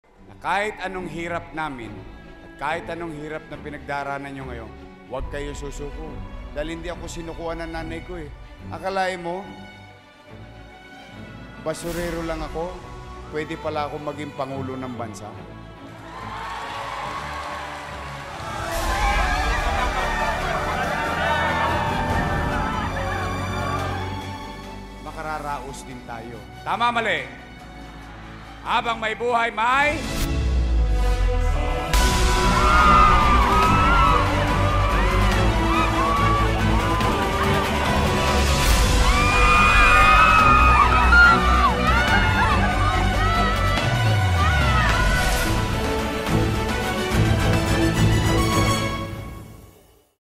Salamat Cagayan Valley sa pagmamahal na ipinakita kay Isko at sa Team Aksyon Demokratiko!
Haan nga agsardeng ni Yorme Isko Moreno Domagoso ti panag-ited ti agpayso nga solusyon ken napartak nga aksyon tapno maitawid ken mairaos amin nga kababayan na idtoy bansa tayo ken adin-no man nga lupalop ti lubong.

Napapanawen na! Ajay nadanas da ijay Manila, madanasan met ti lubong!